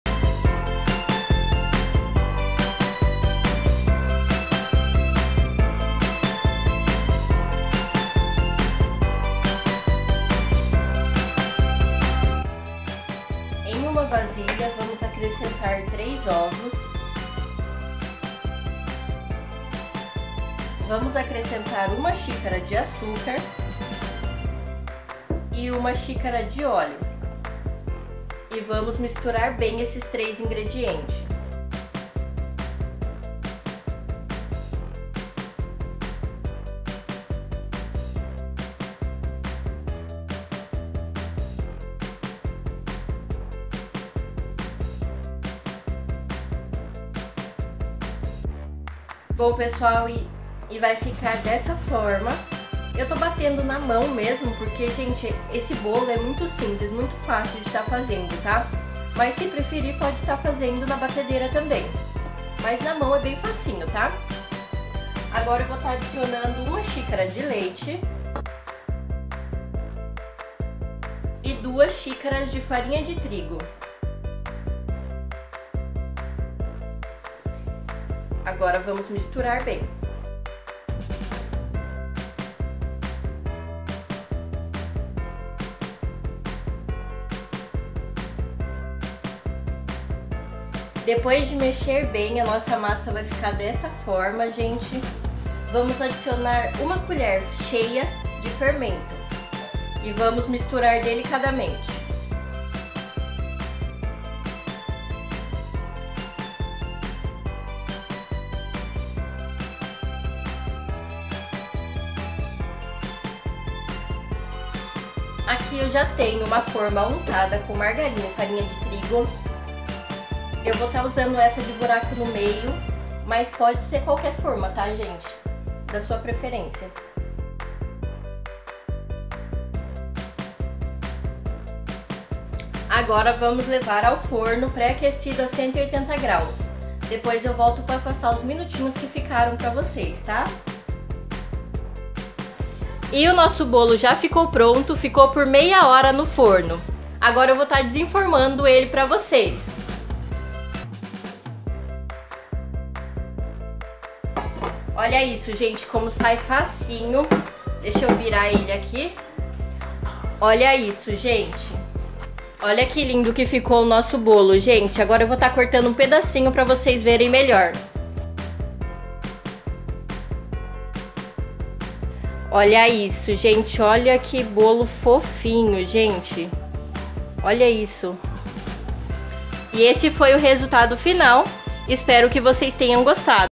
BOLO SIMPLES PARA SEU CAFÉ DA TARDE
Oi gente tudo bem? no vídeo de hoje vim trazer uma receitinha deliciosa de bolo simples, espero que gostem.

os ingredientes são:

3 ovos
2 xícara de farinha de trigo
1 xícara de açúcar
1 xícara de leite
1 xícara de óleo
1 colher de fermento